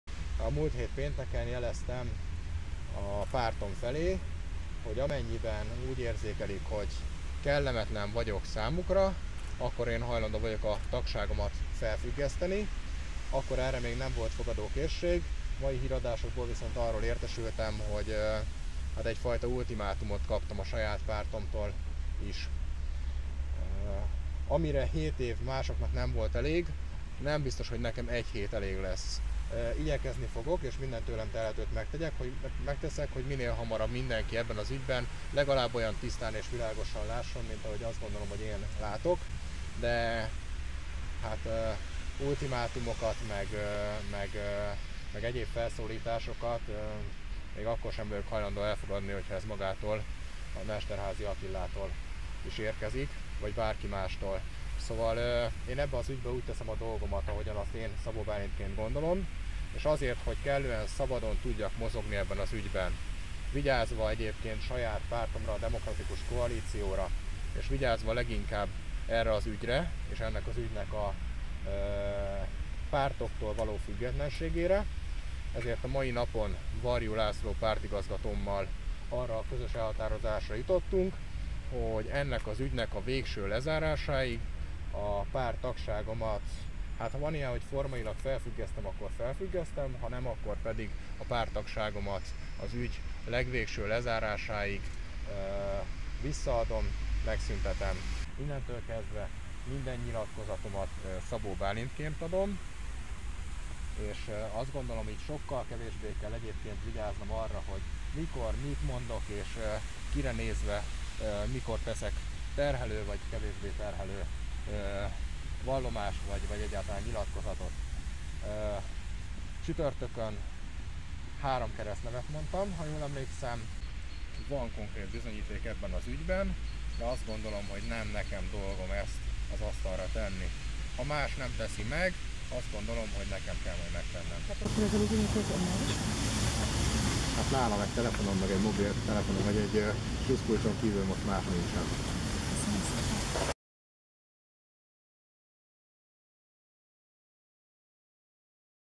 Szabó Bálint felfüggesztette DK tagságát
Először a Heti Válaszban beszélt Szabó Bálint, a Fiatal Baloldal korábbi Vas megyei elnöke, a Gyurcsány Ferenc vezette Demokratikus Koalíció (DK) tagja a hetilapnak az őszödi beszéd...